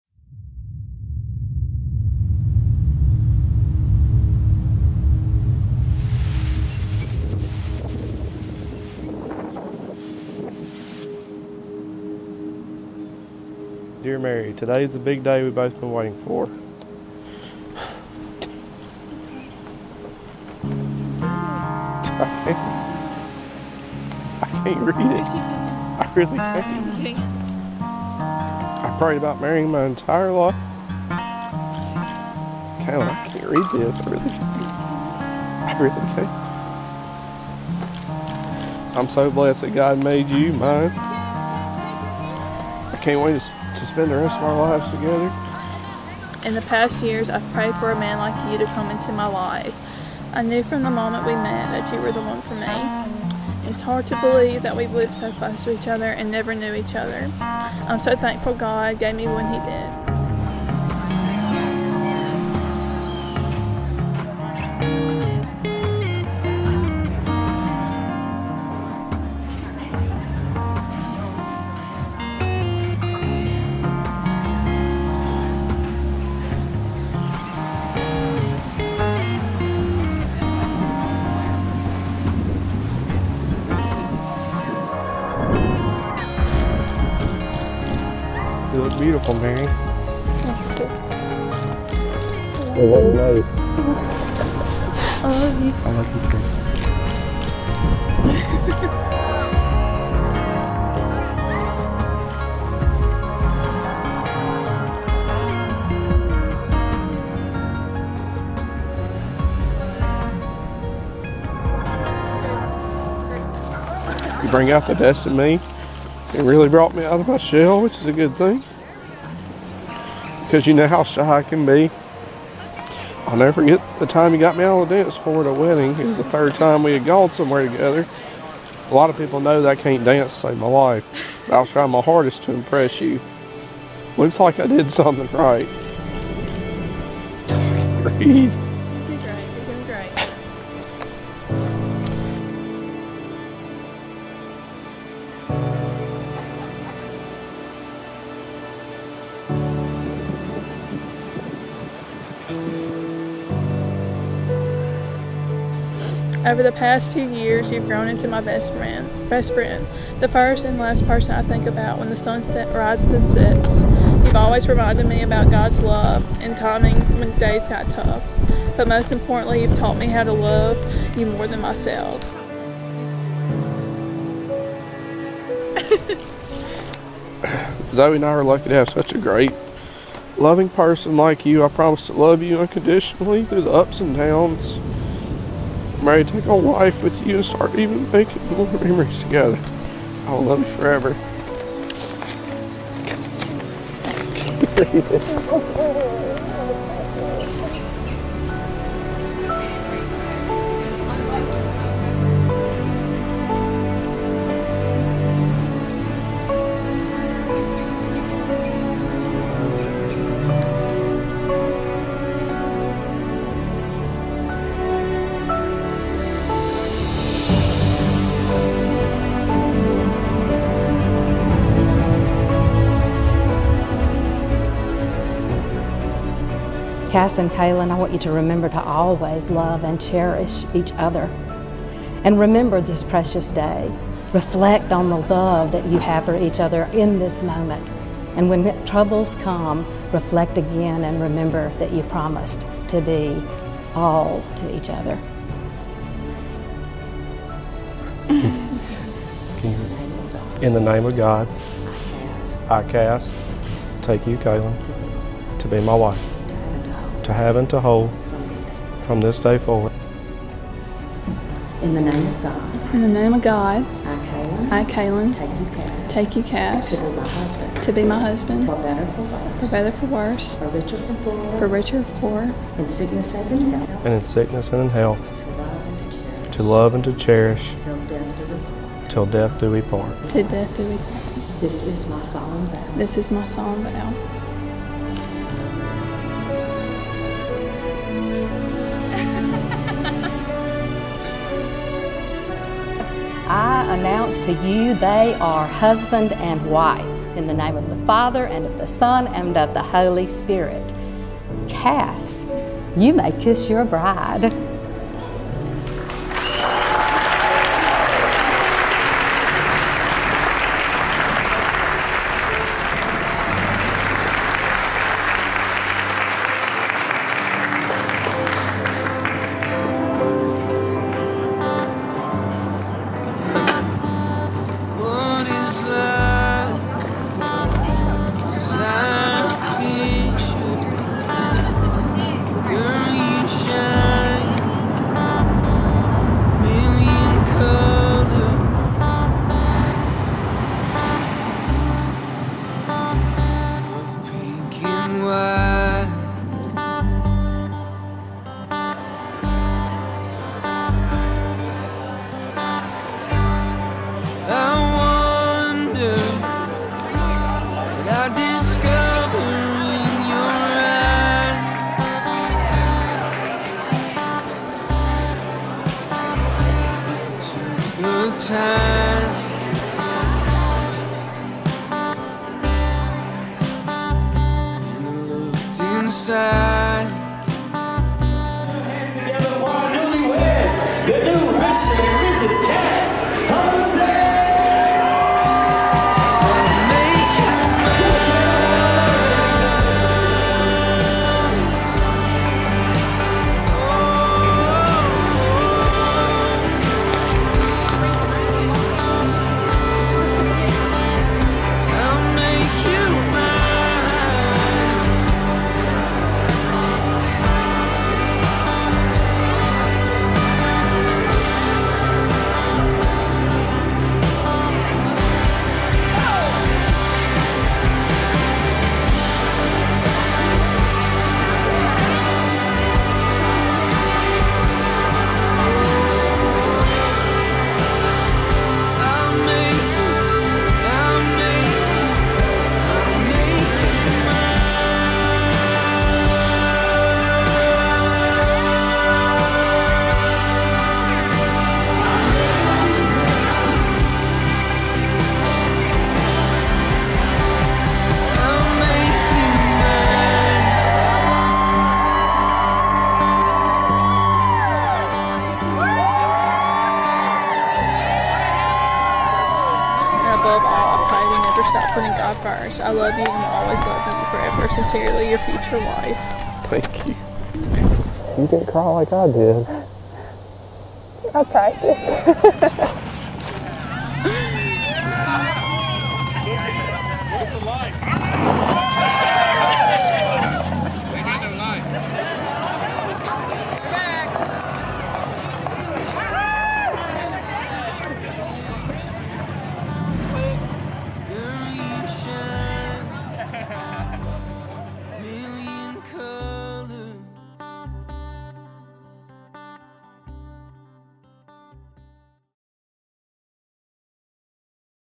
Emotional Letters from the Sweetest Couple | MS Delta Wedding
Caylen & Cass - Highlight | Inverness, MS

There’s truly nothing more lovely than a breezy October wedding day.  With golden leaves falling and the cloudy Autumn sky, Caylen and Cass chose the perfect day to get married. Caylen got ready at her aunt's house in the quaint, but the adorable town of Inverness. The church where the ceremony was held was right across the street. Caylen looked so beautiful in her strapless ball gown wedding dress. Cass looked like a classic gentleman. During their first look, they read letters to each other. This was probably one of our favorite moments in all the many weddings we have had the honor to film. Cass was so blown away by Caylen and the fact that they were getting married that he had to muster up all the control he had to even read his letter to her. Many times he said that he just couldn’t read it with tears in his eyes, all the while Caylen just kept encouraging him that he could. And we are so glad he did make it through because their letters were so touching and heartwarming. Caylen also had a first look with her dad. The ceremony was beautiful. The colors of the old church and the words spoken by the officiant made such an incredible pinnacle of the whole day. The reception was held at the Heathman Plantation in Indianola. The finger foods and hors d’oeuvres were delicious and no one had to wait to get out on the dance floor. Caylen and Cass enjoyed mingling with friends and family. It was the perfect way to end a perfect wedding day. They left the sparkler exit in a vintage red convertible.Caylen and Cass were married on October 22, 2022, at Inverness United Methodist Church in Inverness, Mississippi.
Ceremony Venue | Inverness United Methodist Church
Reception Venue | Commissary Heathman Plantation
Photography | Skelton Photographie
Videography | Abundant Films
Bridal Shop | Amelia Grace Bridal
Tux | Country Gentleman
Hair & Make-up | Devan Ellington
Florist & Caterer | The Pantry Inc.
Band | Style Entertainment
Brides Cake | Shelaine Fulton
Grooms Cake | Cindy Burge
Invitations | Rebekah Caraway
MB01T5SBIPY3YEF